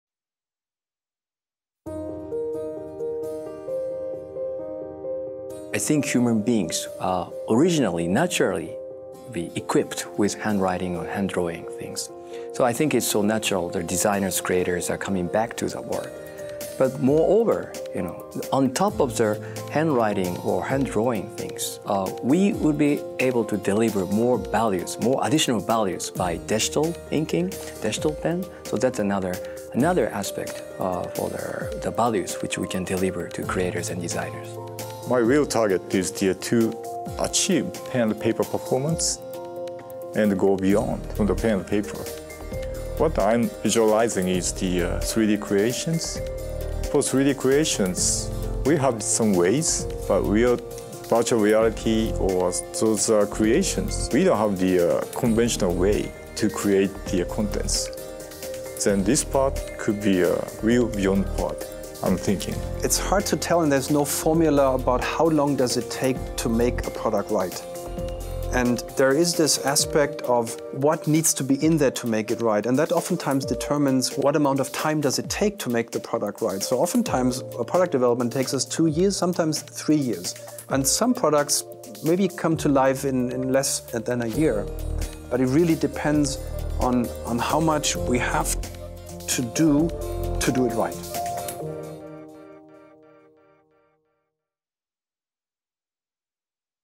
Wacom Talks - Design Philosophy
Wacom CEO Nobu Ide and the engineering/ design team answer the question "How is our design philosophy driving the products we design?"

Topics covered are:
Technology leadership
Driving innovation and new Technologies
The design is more than just looks
Handwriting is the most intuitive input form
Leading design idea for Cintiq